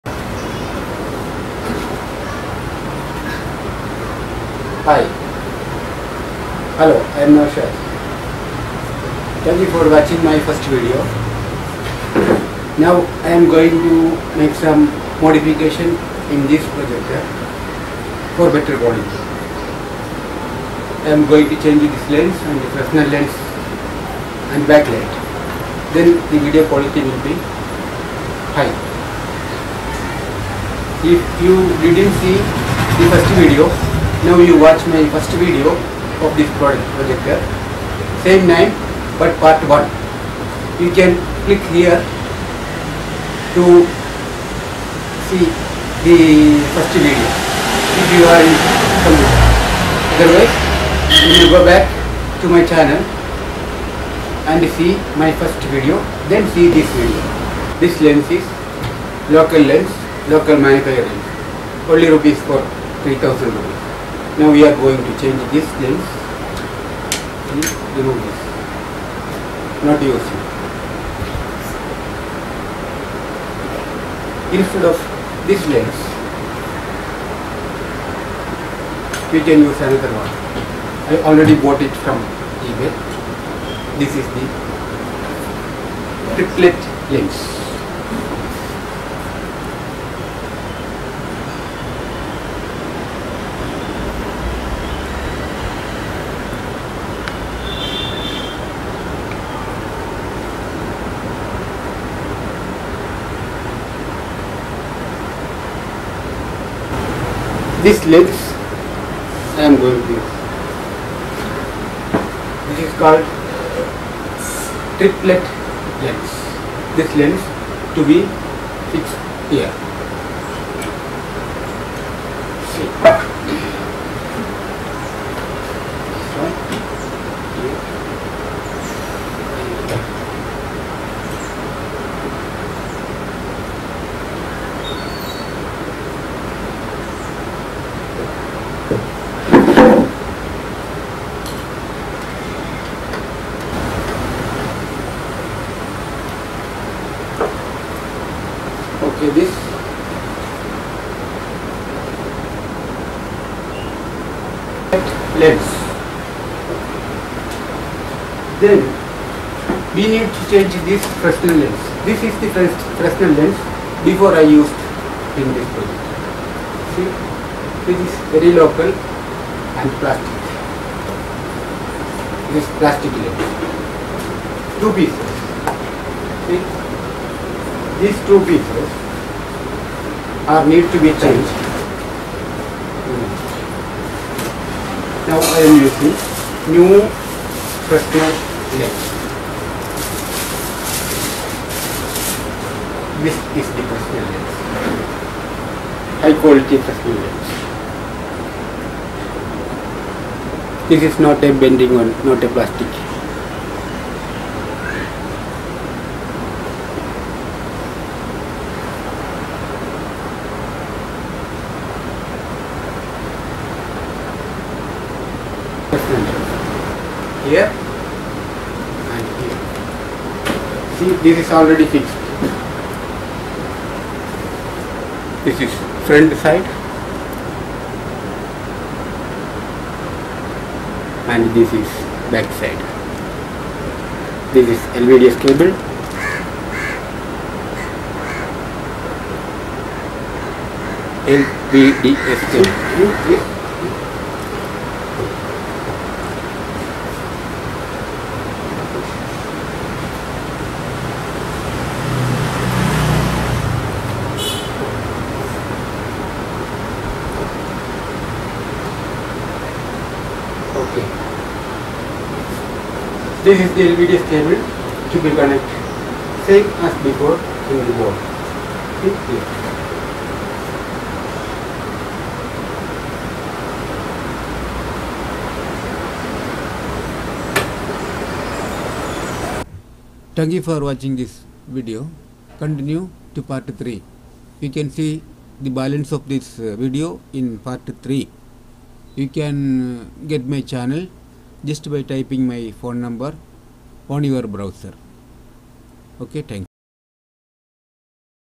HOW TO MAKE A VIDEO PROJECTOR AT HOME PART-2 ( HD Movie Theater: LED Video Projector )
LED / LCD TV service Course
LED/ LCD installation
HD Projector
CCTV installation
Laptop Service etc.